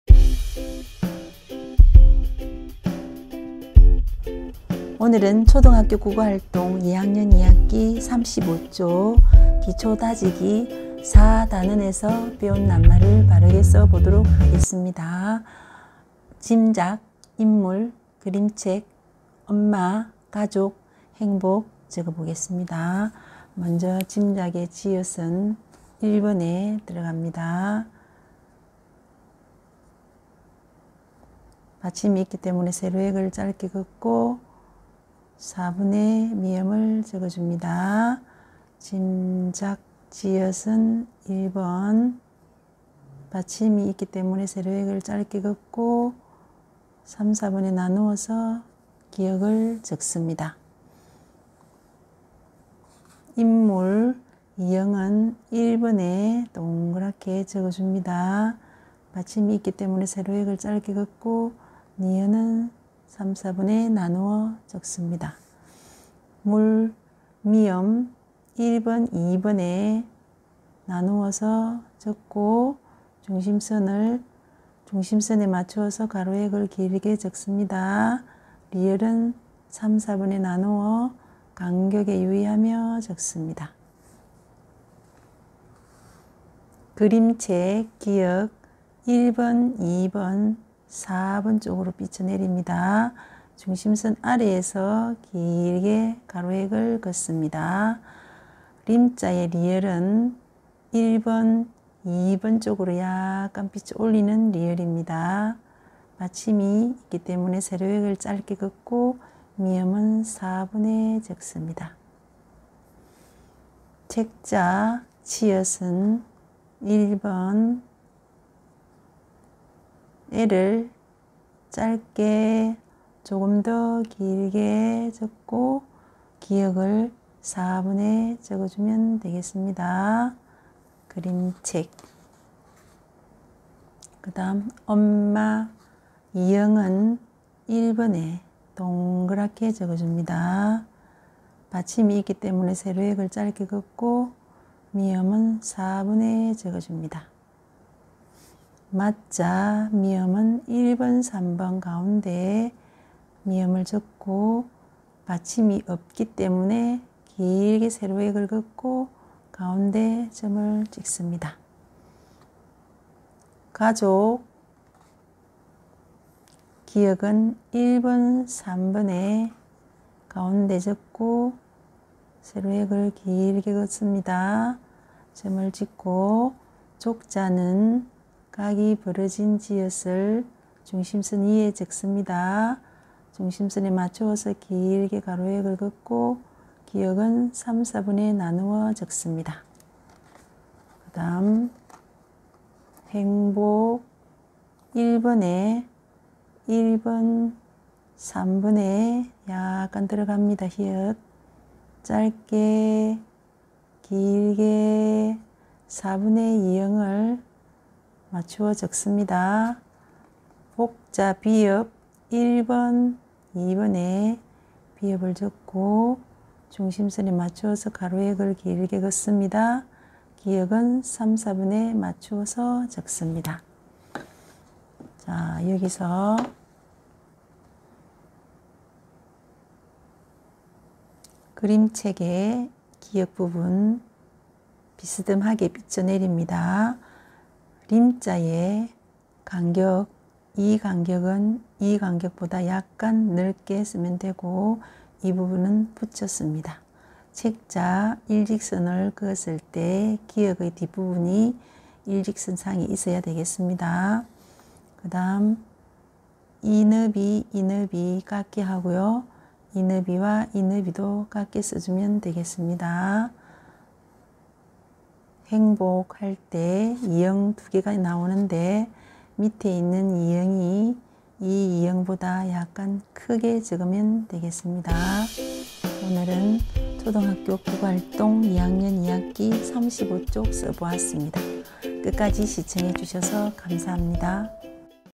[악필교정 방법]초등학교 국어활동 2학년 2학기 35쪽 기초 다지기, 4단원에서 공부한 낱말 바르게 쓰기
이 영상은 초등학교 국어활동 글씨 쓰기 내용이지만 악필 때문에 고민이신 성인들과 학생을 지도하시는 선생님, 가정에서 자녀에게 글씨 쓰기를 지도하실 학부모님들을 위한 글씨 쓰기 지도 자료입니다. 도움이 되시길 바랍니다.
화질이 선명하지 못한 점 양해 바랍니다.
색연필: 스테들러 노리스클럽 코끼리 색연필 1287

글씨 쓰기에 어려움을 느끼시는 분들을 위해
속도를 배속으로 보실 수 있게 되어 있으니 참고 부탁드립니다.
Elementary school Korean language activity 2nd grade 2nd semester, page 35, writing letters with support correct writing
Hello?
even realizing.